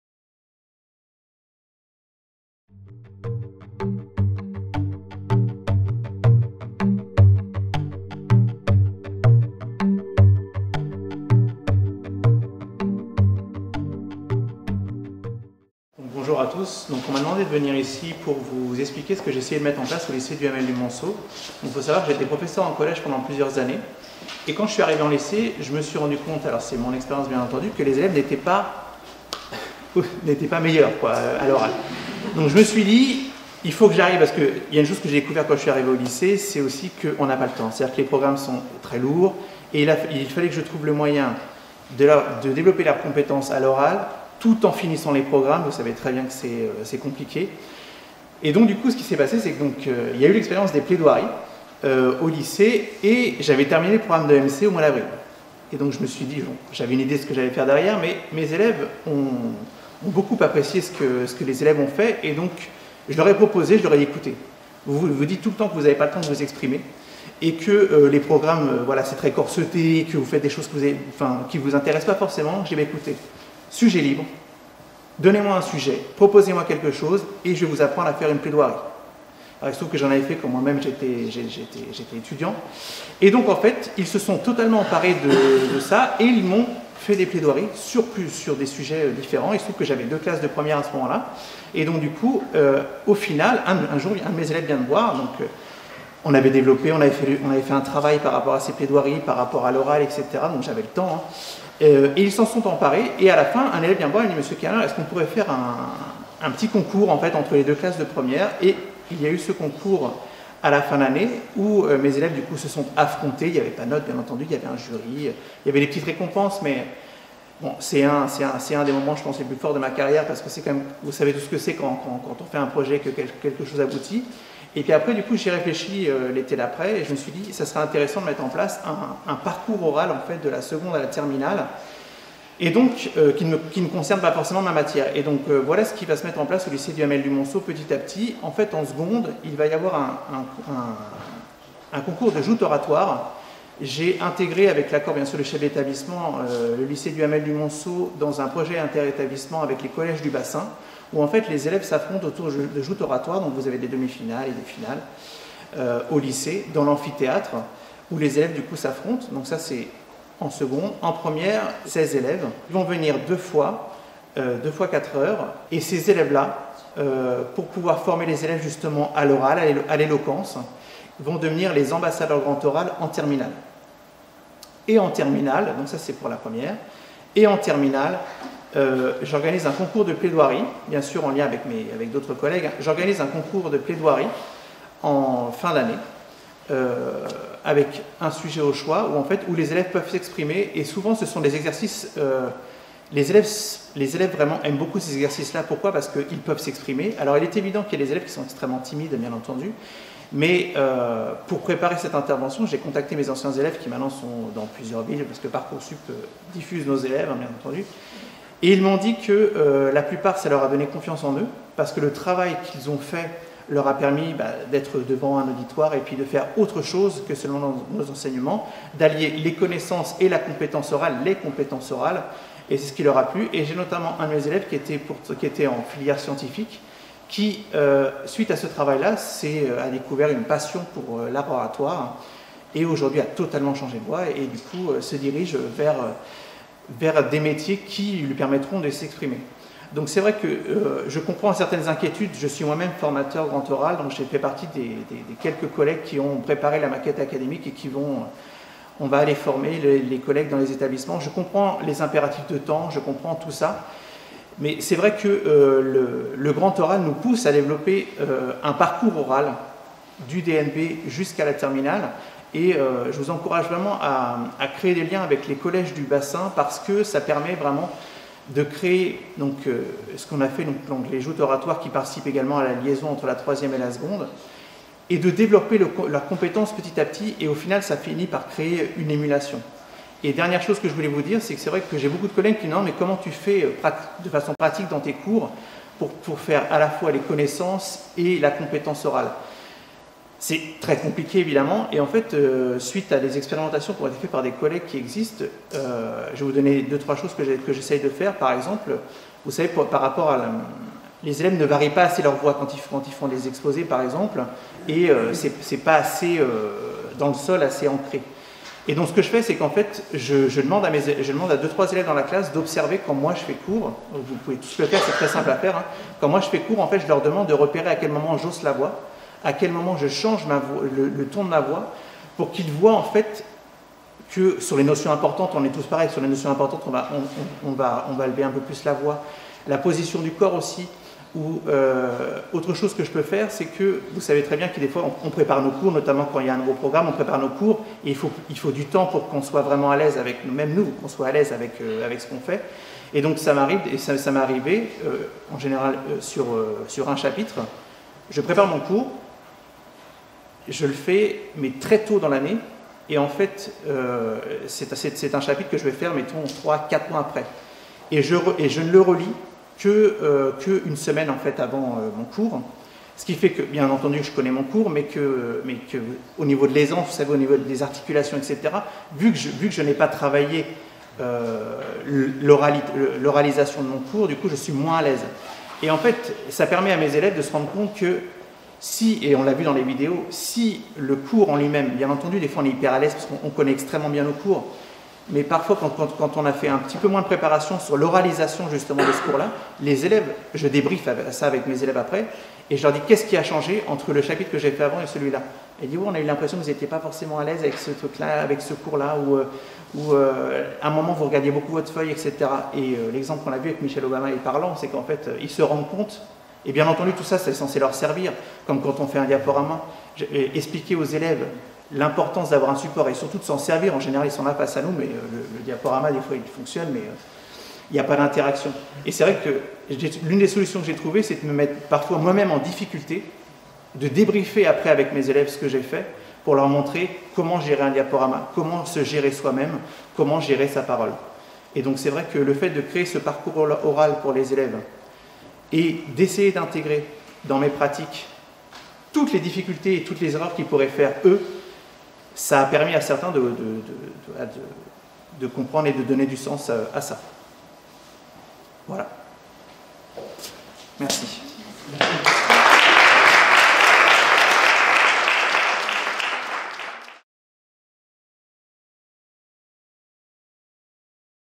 Quelques pistes pour les enseignants
Dans cette vidéo :

M. Kerneur, professeur d’histoire-géographie au lycée, présente quelques pistes aux enseignants, pour permettre aux élèves de développer des compétences orales.
• L’enseignant évoque d’abord l’expérience de plaidoiries réalisées par des lycéens sur un sujet libre.
• Il présente ensuite trois temps d’un parcours oral au lycée, ce parcours pouvant démarrer au collège et contribuer à la liaison collège-lycée.
• Il termine par évoquer le travail de l’oral en classe, en s’intéressant à la fois aux compétences langagières et aux compétences disciplinaires. Il cite quelques pistes de mise en situation du professeur, en évoquant un effet miroir pour les élèves.
Florian KERNEUR, professeur d’histoire-géographie au lycée Duhamel du Monceau à Pithiviers